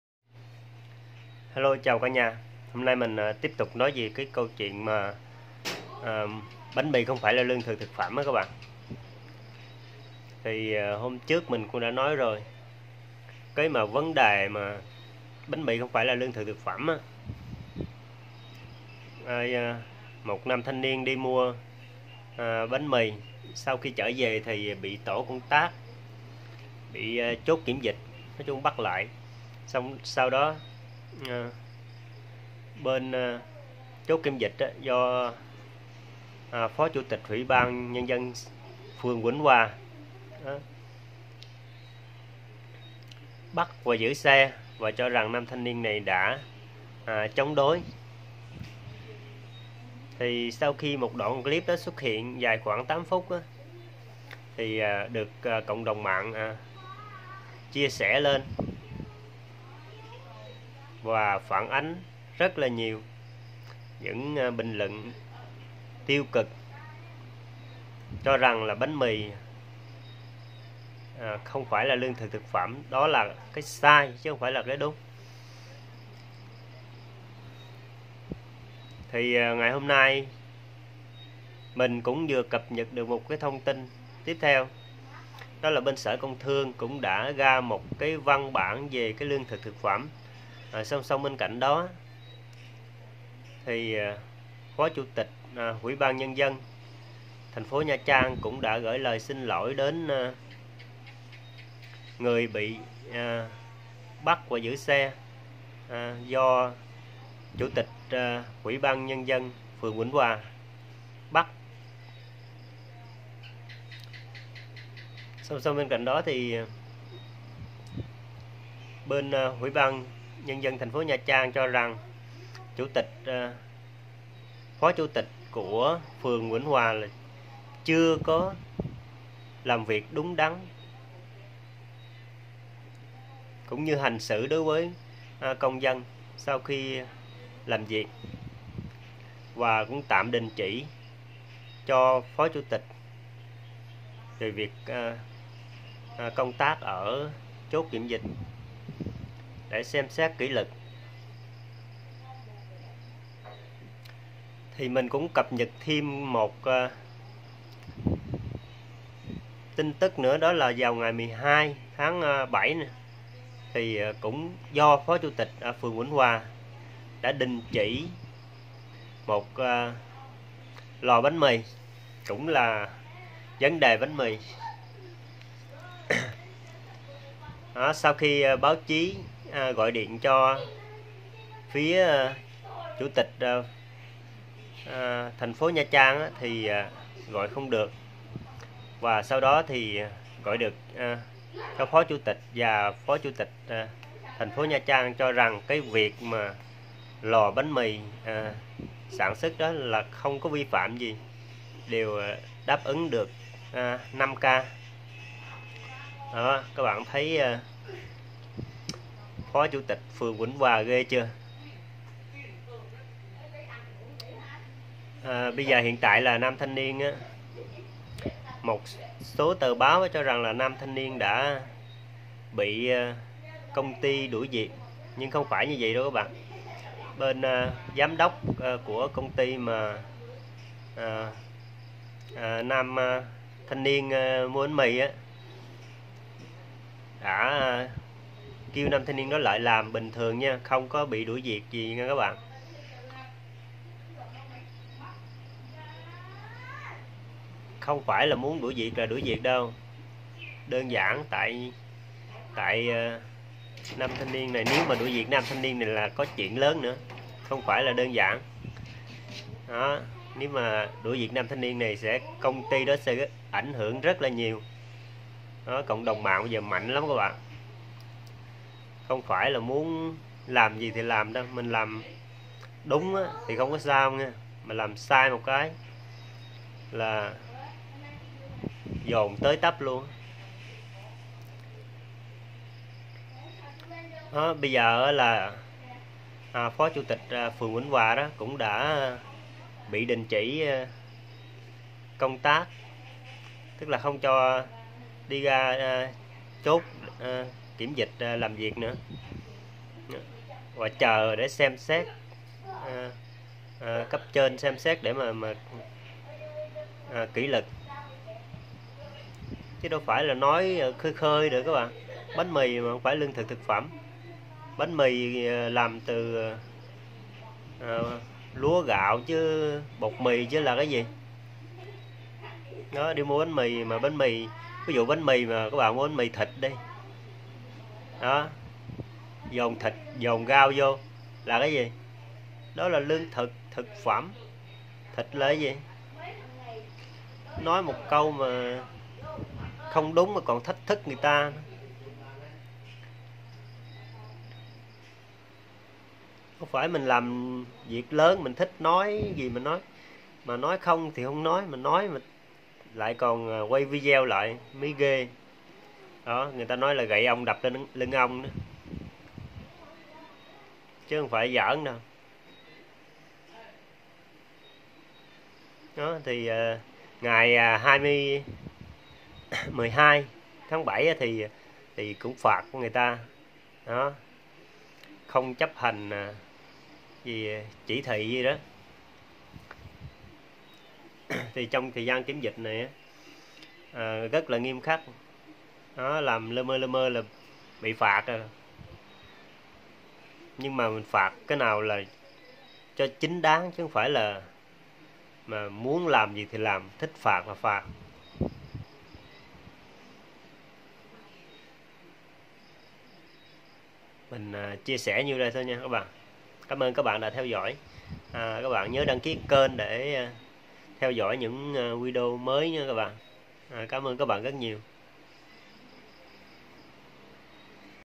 CÂU CHUYỆN BÁNH MÌ KHÔNG PHẢI LÀ HÀNG THIẾT YẾU /Anh Ni official
Liên quan đến vụ bánh mì không phải là lương thực thực phẩm , hôm nay  chủ tịch ủy ban nhân dân TP nha trang đã gửi lời xin lỗi đến nam thanh niên bị phó chủ tịch phường Vĩnh Hòa bắt và gửi phương tiện...
Cảm ơn anh chị cô chú và các bạn